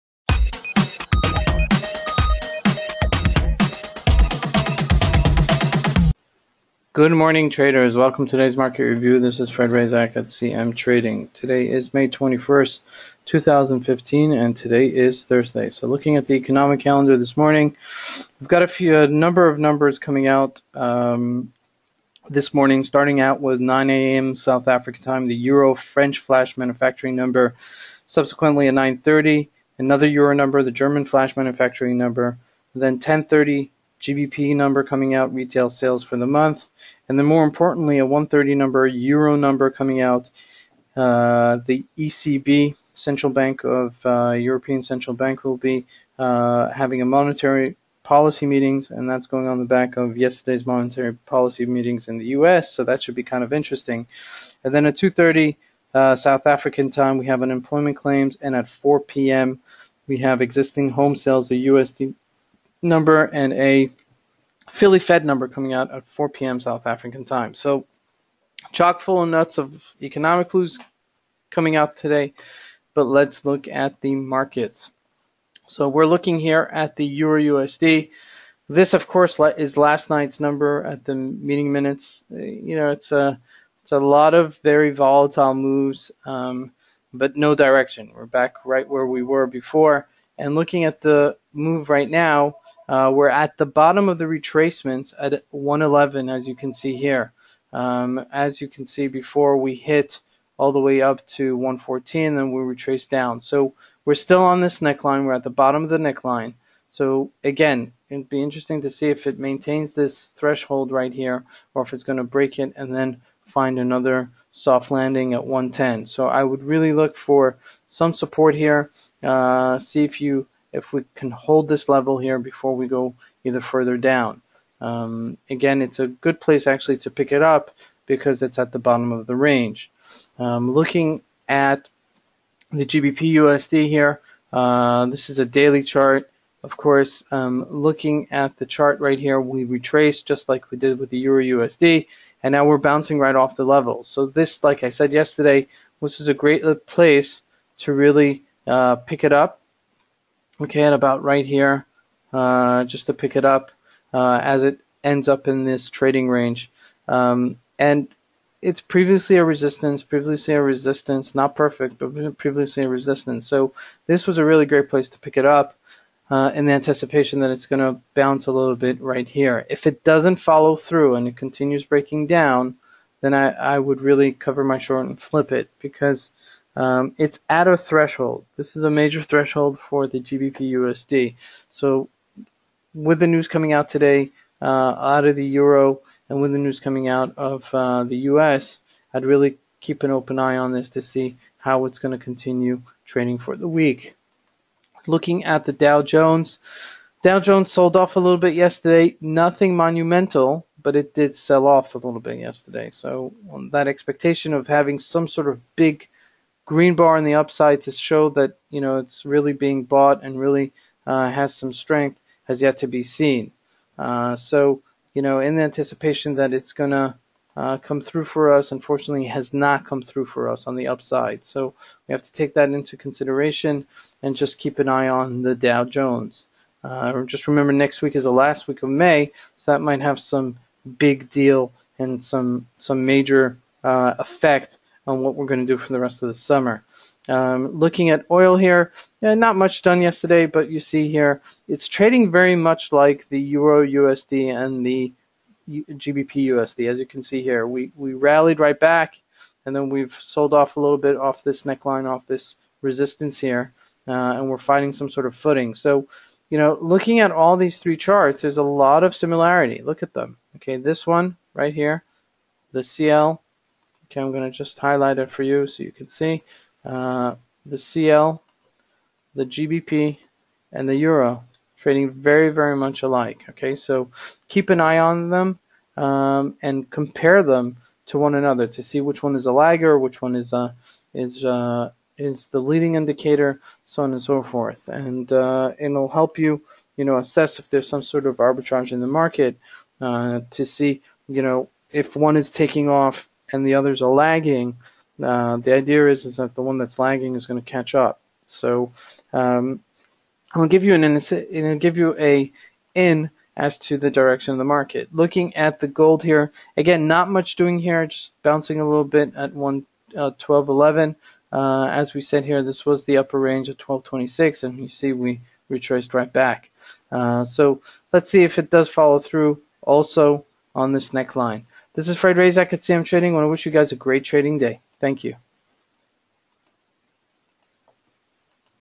CMTrading - Daily Forex Market Review 21.05.2015
Morning Review by CMTrading's Top Analyst Fred Razak.
Please follow Fred on YouTube, Facebook and Google+ for exclusive market analysis